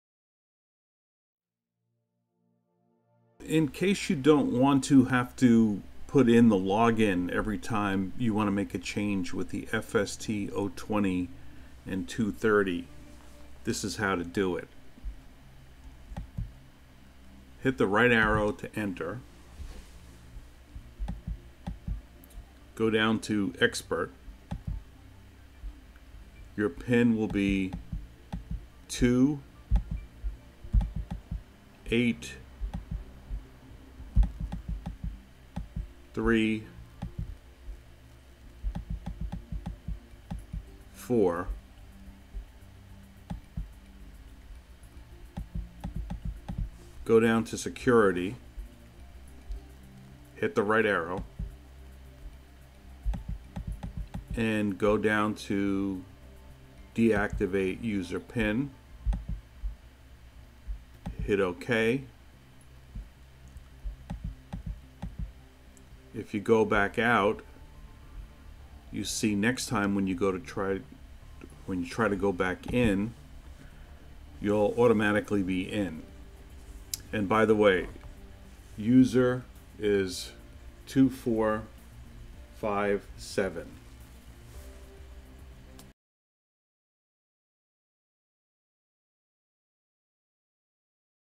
Disabling the user pin for the SITRANS FST020 and SITRANS FS230
In this video, Siemens will show you how to disable the user pin to the SITRANS FST020 and SITRANS FS230.